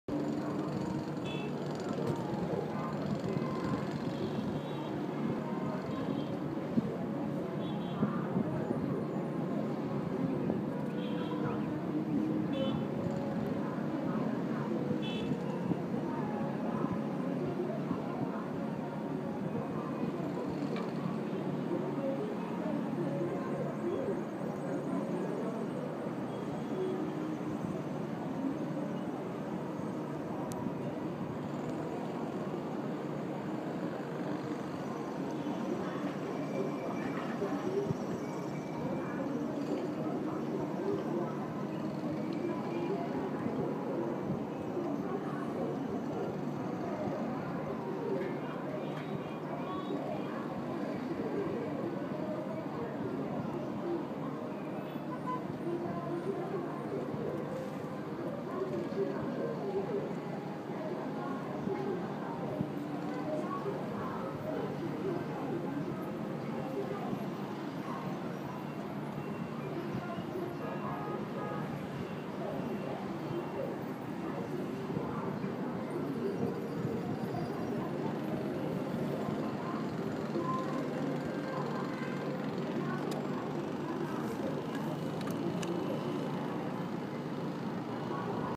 Jiaozuo, China from my hotel window
As captured by iPhone wavering on a shaky palm jostled out thru an opening in the window of my hotel room on 22nd floor :)